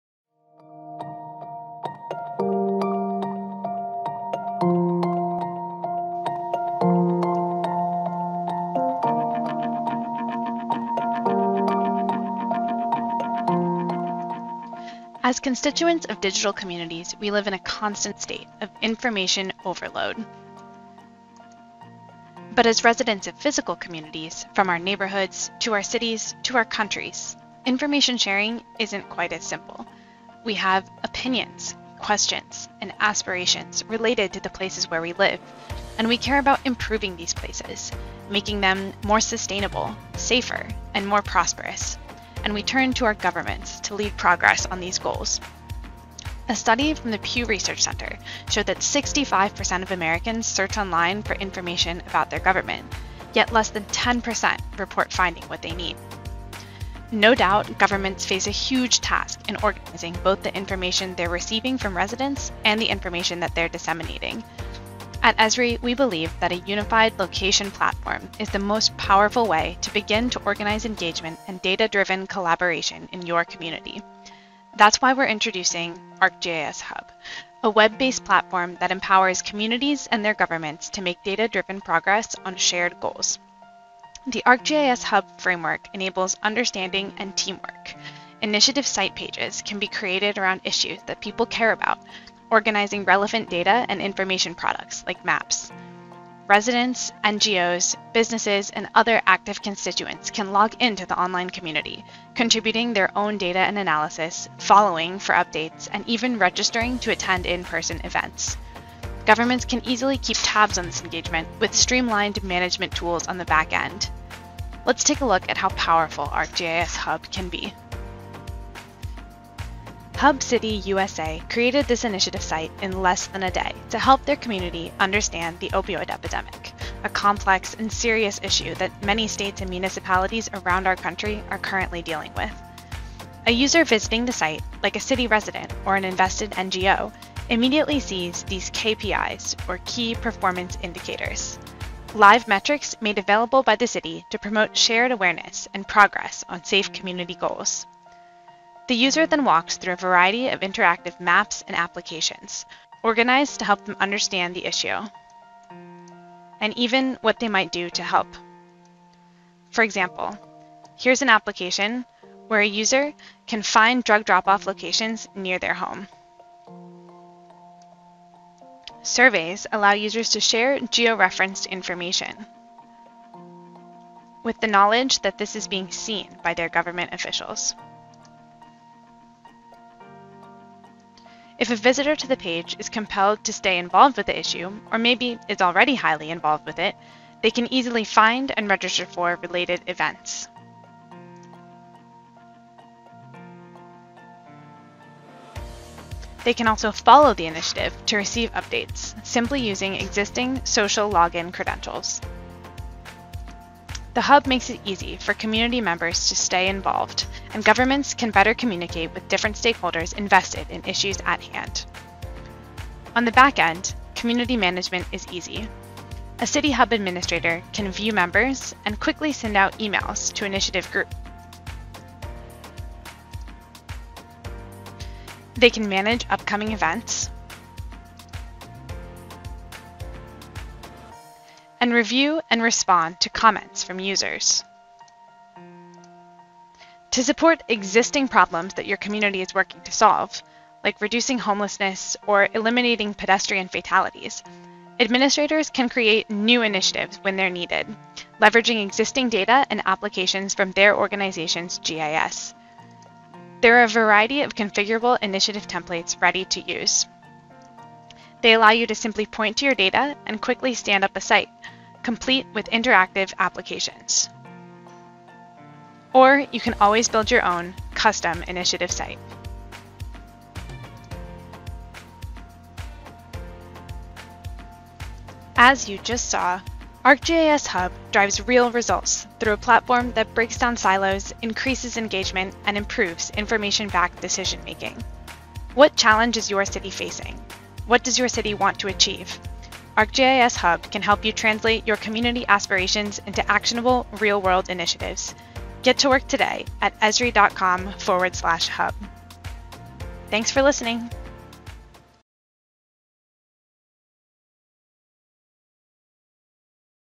ArcGIS Hub
Conoce ArcGIS Hub, una nueva manera de colaboración entre los municipios y sus comunidades. Aprende cómo las municipalidades pueden usar los datos abiertos y la transparencia para conectarse con su población en asuntos cruciales, desde la indigencia hasta la seguridad vial.